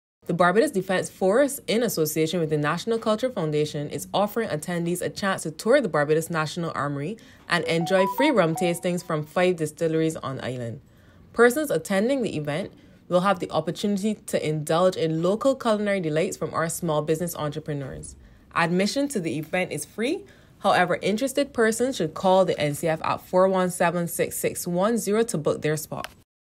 Barbados National Armoury Tour and Rum Tasting comes off today
Barbadians will have a special opportunity to stroll down memory lane today when the Barbados Defence Force (BDF) presents the Barbados National Armoury Tour and Rum Tasting in association with the National Cultural Foundation.

NCF Marketing Officer Ashley Dyall told CBC News from 2:00 p.m. to 7:00 p.m., the historic St. Ann's Fort will be transformed into a hive of activity.

Each patron will be allocated tokens to sample the rums available from Mount Gay Rum, Plantation Rum, St. Nicholas Abbey Rum, Foursquare Rum and Bajan 1966 Rum.

Specialty attractions include the Barbados Tourism Marketing Inc. Accelerator Programme, which will showcase locally made food products.

Also on hand will be a traditional coconut vendor with branded coconuts to celebrate the Independence season, as well as a snow cone cart loaded with rum mixes and Bajan syrups.